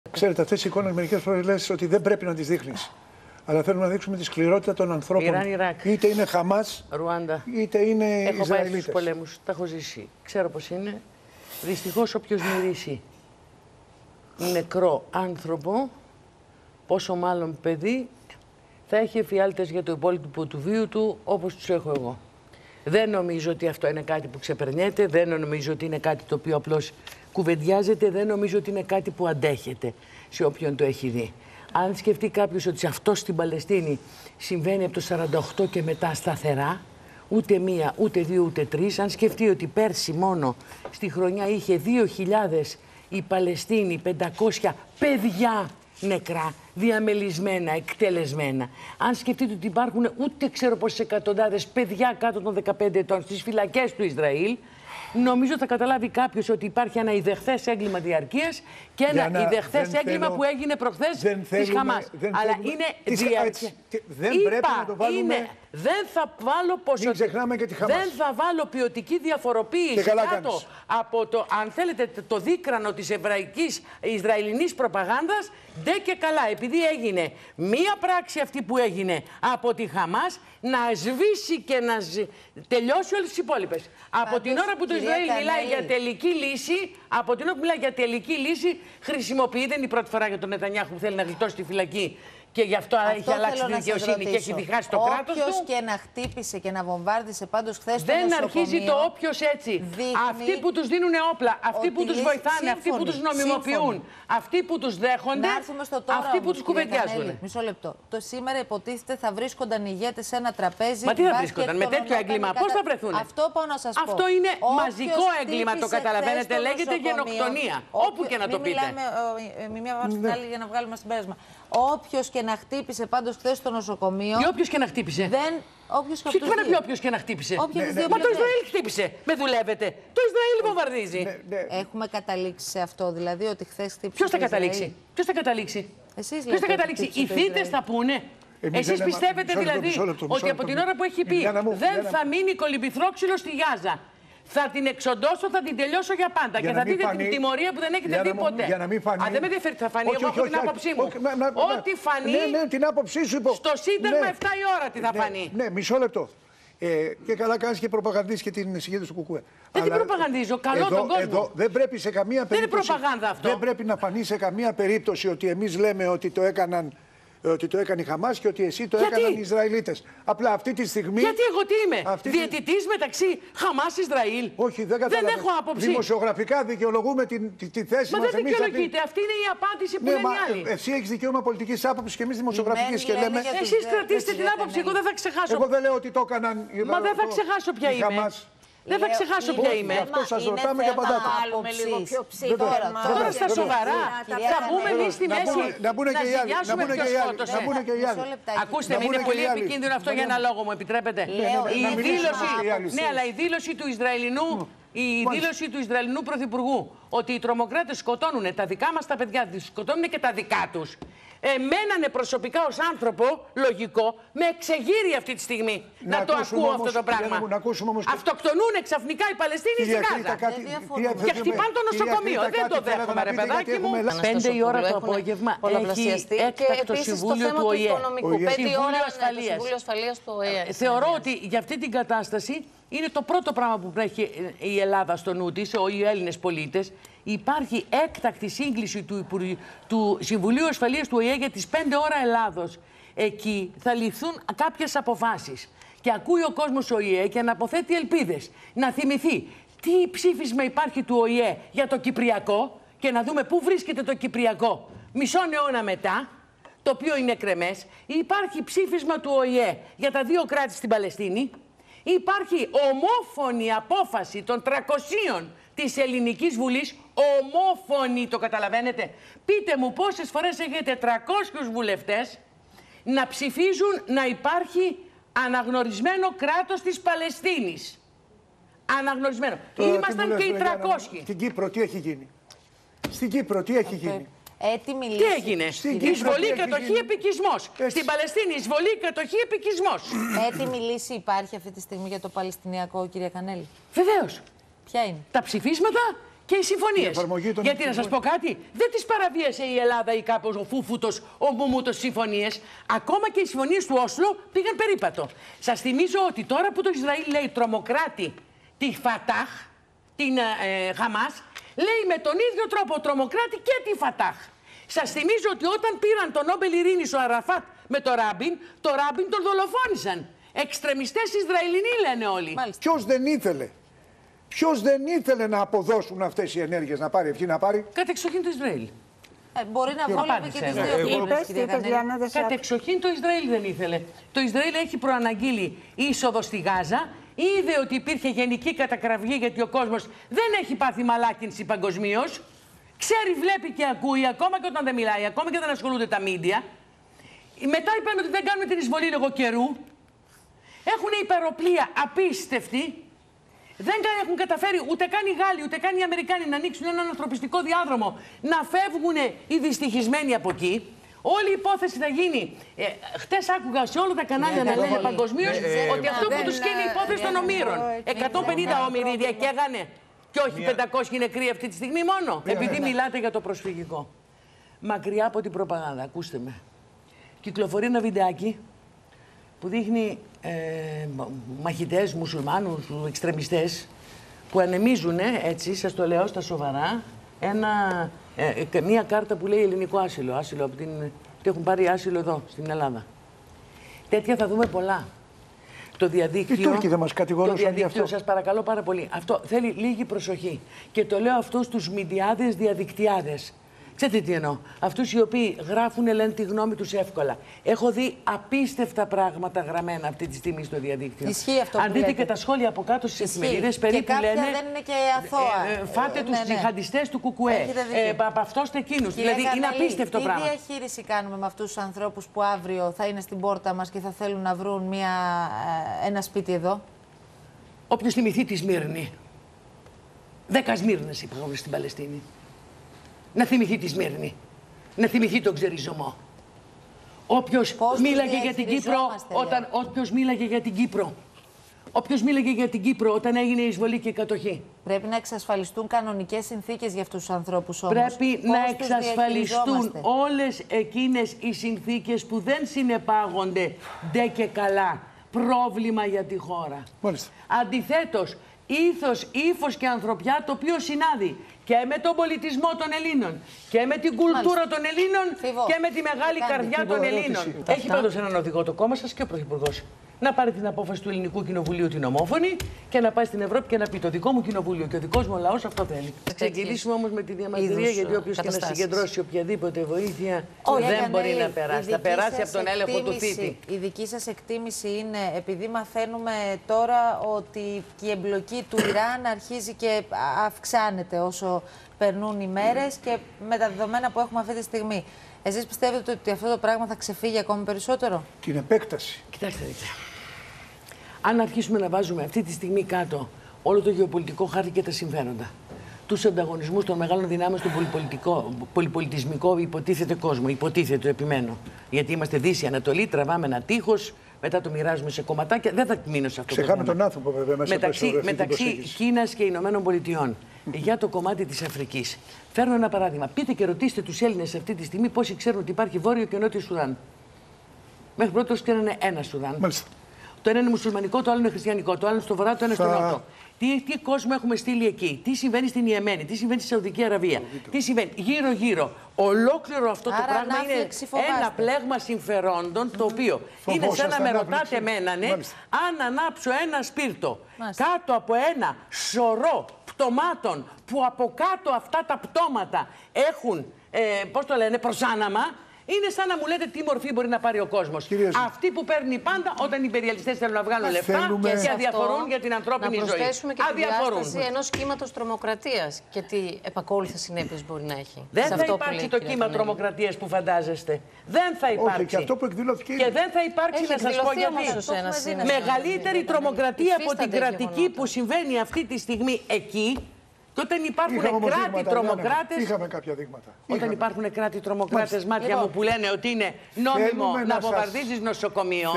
Λ. Κανέλλη: Μαζικό έγκλημα και γενοκτονία του κράτους του Ισραήλ σε βάρος του λαού της Παλαιστίνης
Το μαζικό έγκλημα στο Νοσοκομείο στη Γάζα και την γενοκτονία του κράτους του Ισραήλ σε βάρος της Παλαιστινιακού λαού κατήγγειλε η βουλευτής του ΚΚΕ, Λιάνα Κανέλλη, μιλώντας το πρωί της Τετάρτης στον ΑΝΤ1, εξηγώντας πως αυτό συμβαίνει από το 1948.

Όπως είπε, μόνο την περσινή χρονιά ο παλαιστινιακός λαός θρήνησε 2.500 παιδιά, ενώ εκατοντάδες είναι οι ανήλικοι κάτω των 15 ετών στις φυλακές του Ισραήλ.

Σχετικά με τον χθεσινό βομβαρδισμό του νοσοκομείου στη Γάζα η Λ. Κανέλλη τόνισε πως πρόκειται για ειδεχθές έγκλημα.

Παραλλήλισε την εισβολή, την κατοχή και τον εποικισμό στην Κύπρο, με αυτή στην Παλαιστίνη, και συνέστησε προσοχή από τον λαό στην παραπληροφόρηση και την προπαγάνδα.

Ακόμα τόνισε ότι πρέπει να εξασφαλιστούν όλες οι συνθήκες για πρόσφυγες που πιθανά να φτάσουν στην Ελλάδα.

Αναφέρθηκε ακόμα στους εντονότατους γεωπολιτικούς ανταγωνισμούς σε Μέση Ανατολή και Αφρική, που αποτελούν πλέγμα συμφερόντων των ιμπεριαλιστών.

Τέλος, θύμισε ότι υπάρχει ομόφωνη απόφαση της Βουλής για αναγνώριση του κράτους της Παλαιστίνης που δεν έχει εφαρμοστεί.